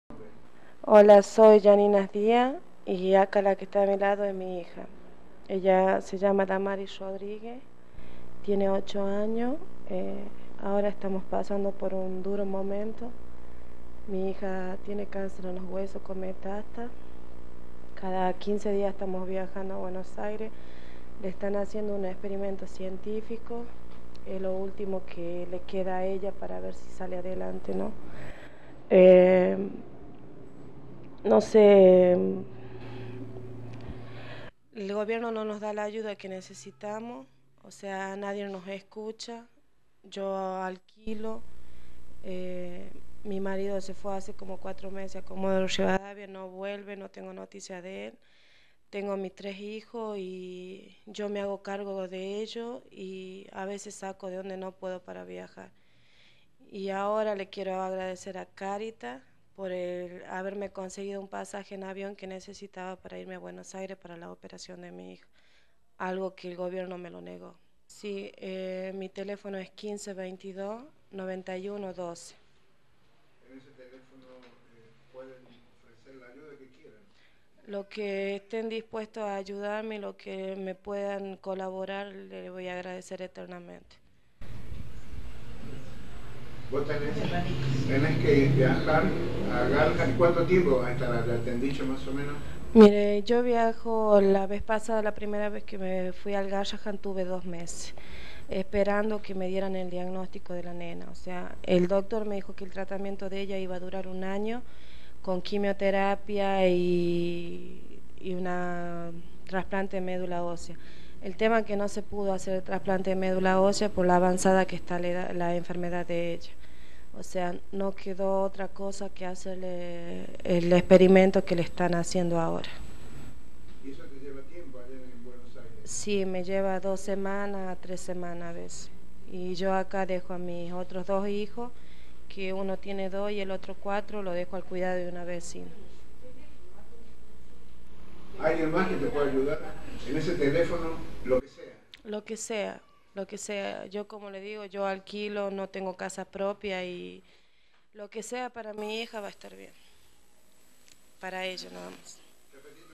NOTIRIOJATV: URGENTE AYUDA
Yanina Diaz la madre de esra niña que debe atenderse en Bs. As. por el tipo de cancer que padece debe trasladarse permanentemente al Hospital Garraham para que los medicos puedan seguir haciendo el tratamiento correspondiente por ello la mamá desea que le puedan ayudar economicamente. Comunicate con Caritas para ayudar.